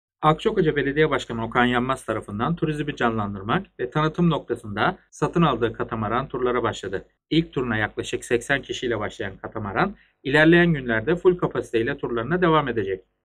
Katamaran Turlara Başladı
Akçakoca’ya renk katan, tanıtım ve reklam konusunda büyük hizmetleri olan katamaran gezi teknesi turlarına başladı.